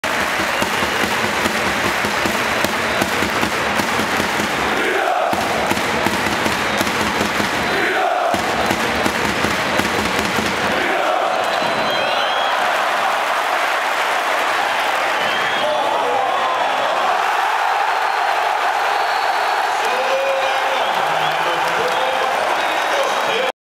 FC Barcelona vs Olympiacos Championship game at the 10th Euroleague Final Four in Paris
EPIC Papaloukas, Theodoris the Olyampiacos guard fake pass lay-up, more